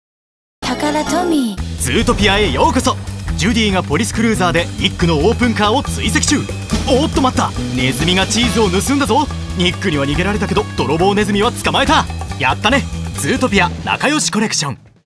ズートピア なかよしコレクション 15秒ver.
. 商品名**ズートピア(おんなのこむけ/おとこのこむけ)**
大人気ディズニー映画「ズートピア」の作中で活躍するビークルや愉快なキャラクターたちのフィギュアたちが登場！なかよしコレクションのフィギュアとビークルは一緒に遊べるよ。みんな集めて映画の再現をしよう！

■ズートピア玩具情報はこちら！